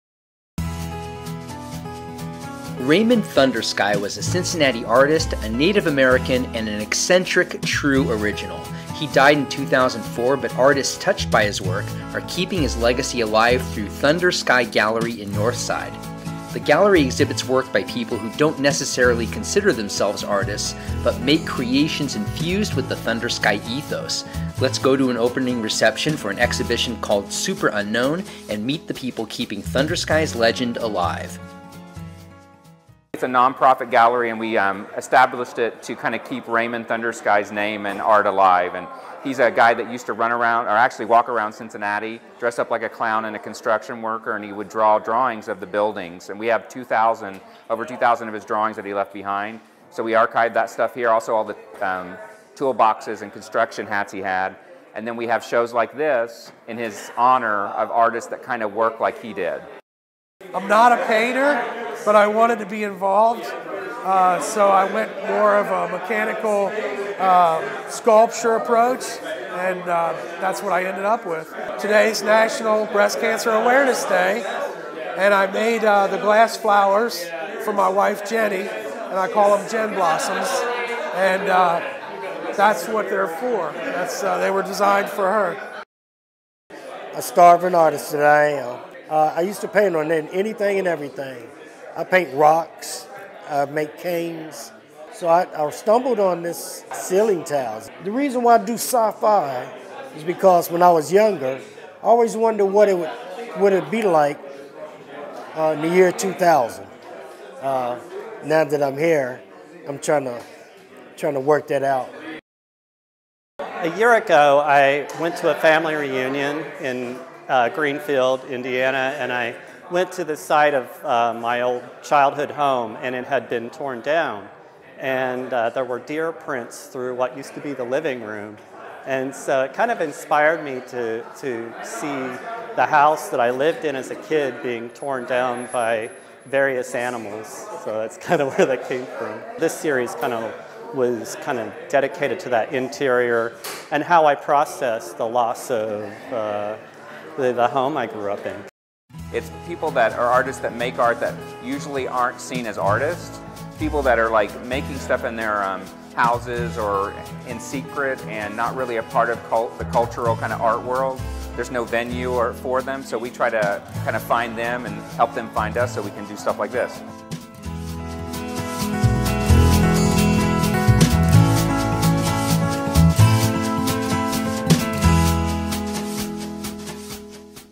Thunder-Sky Gallery pays homage to an iconic, eccentric artist
This video profiles Thunder-Sky Gallery in Northside, which preserves the work of Cincinnati artist Raymond Thunder-Sky and exhibits local artist who embody Thunder-Sky's street artist ethos.